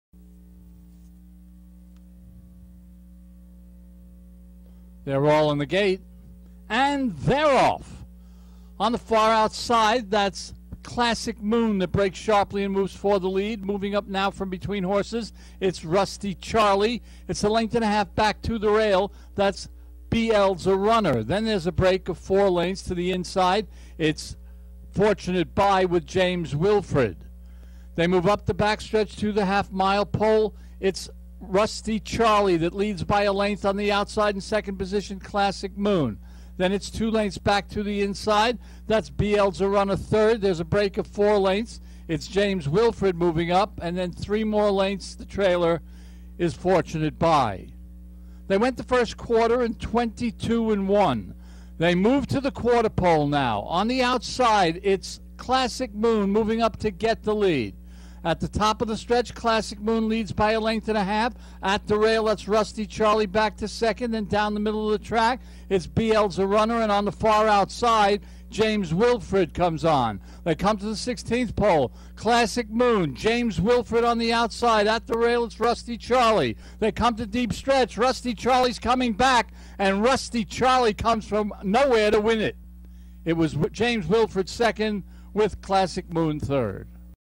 CALDER RACE COURSE, 2009-07-04, Race 10
Entries: Thoroughbred Race, CALDER RACE COURSE, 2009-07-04, Race 10:
      (Pgm-Runner-Jockey-Finish)
       1A-Classic Moon (FL), Cruz, M, finished 3;
       2-B L's a Runner (FL), Vigil, N, finished 4;
       3-Fortunate Buy (FL), Smith, A, finished 5;
       5-James Wilfred (FL), Leyva, J, finished 2;
       6-Rusty Charlie (FL), Colon, L, finished 1;